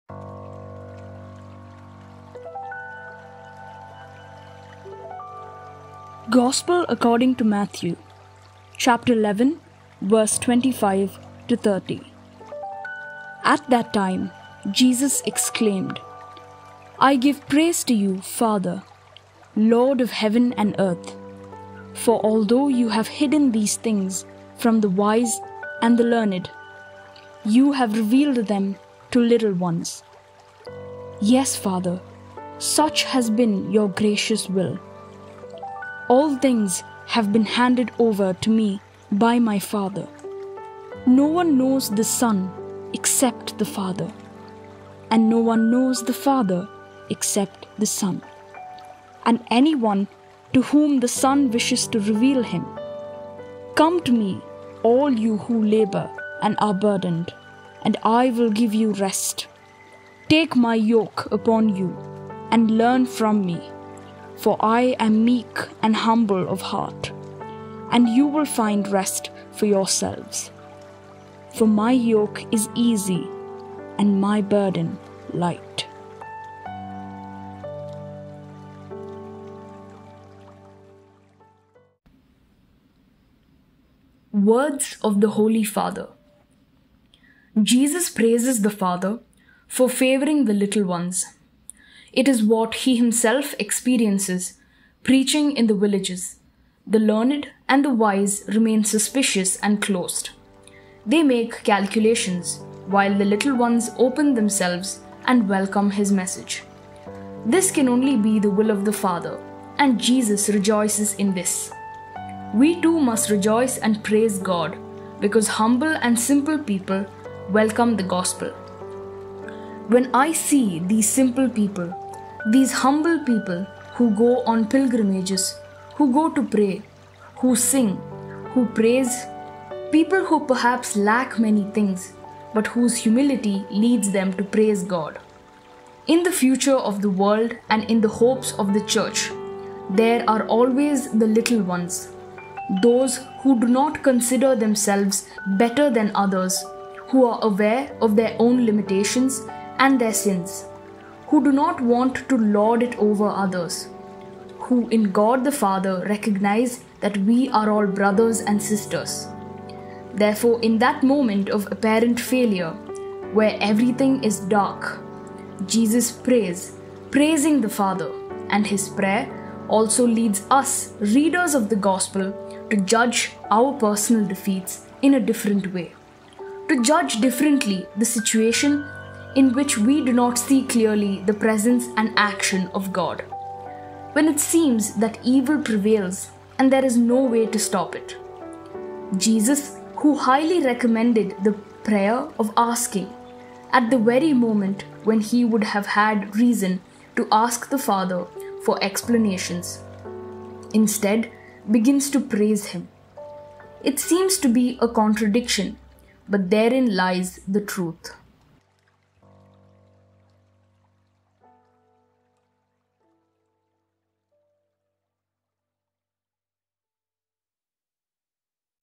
Matthew 11:25-30 ( June 16, 2023 ) Gospel Reading & Reflection | Solemnity of Sacred Heart of Jesus
Today's Catholic Bible Reading & Gospel Reflection for June 16th 2023 , the Friday of the Tenth Week in Ordinary Time Year A

Solemnity of the Most Sacred Heart of Jesus

Mt 11:25-30

At that time Jesus exclaimed:
"I give praise to you, Father, Lord of heaven and earth,
for although you have hidden these things
from the wise and the learned
you have revealed them to little ones.
Yes, Father, such has been your gracious will.
All things have been handed over to me by my Father.
No one knows the Son except the Father,
and no one knows the Father except the Son
and anyone to whom the Son wishes to reveal him.

"Come to me, all you who labor and are burdened,
and I will give you rest.
Take my yoke upon you and learn from me,
for I am meek and humble of heart;
and you will find rest for yourselves.
For my yoke is easy, and my burden light."